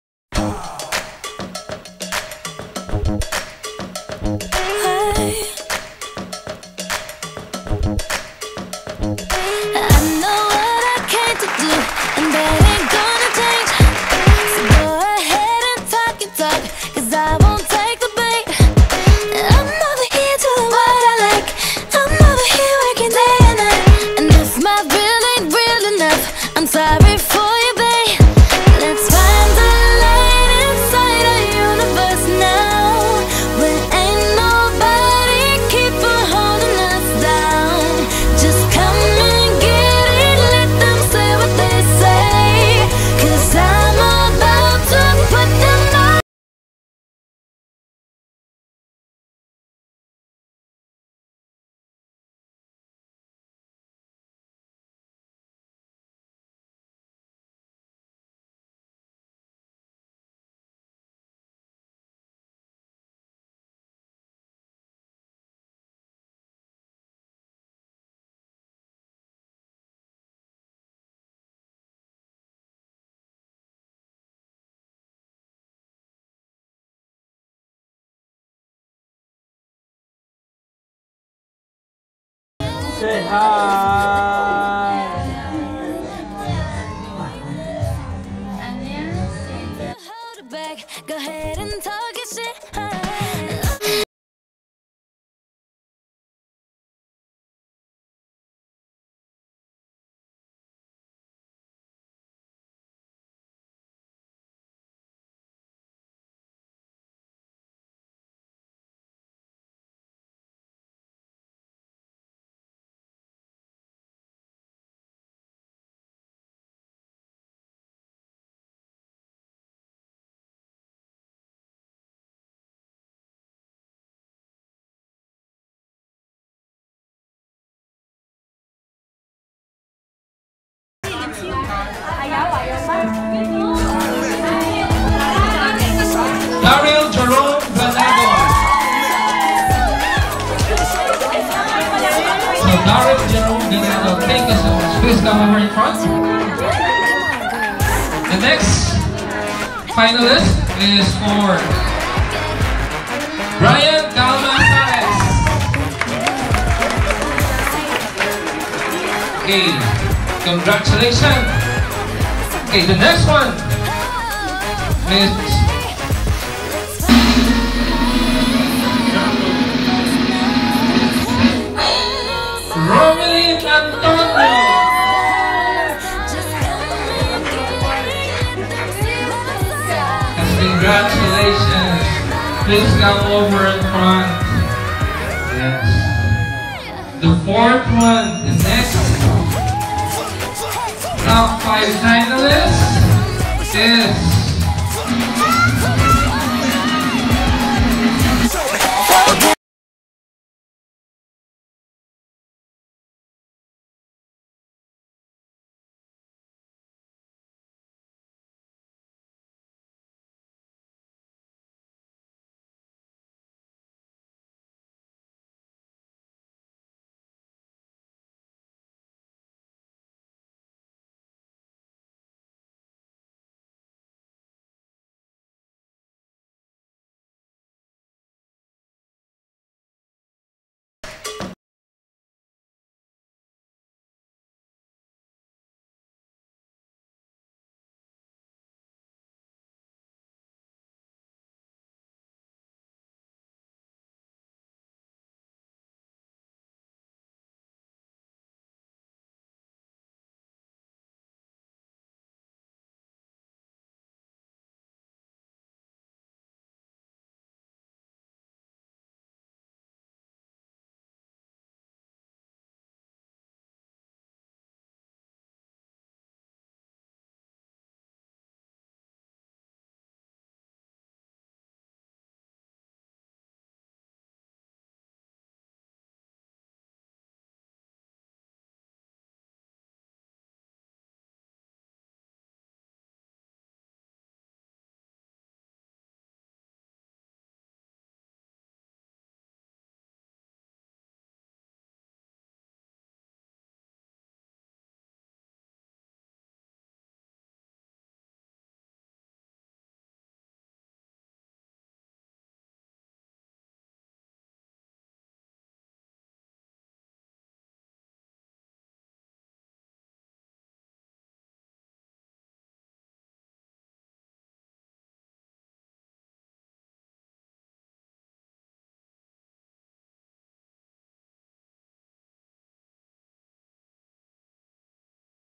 My Experience In Joining A Competition-Battle Of The Brushes 2018
This is so late upload but still I want to share it again...
I want to share this award to my family and friends especially to our one and only son @alken antonio, to my supporters during the competition @hikari and @akime and to my ever supportive husband @joel antonio, to my dear model during the competition my dear @victoria who’s very patient and ever supportive and to ate @jenny lynn thank you so much for the kindness. To the person behind that beautiful dress which my model victoria is wearing: Ms. @Gweys and Mr. @Khel thank you so much 😊🤗
I will never forget this experience ever. Thank you so much Papa God.

Please also like, follow and share my:
Facebook Page: One4Beauty Makeup Artistry by Lyn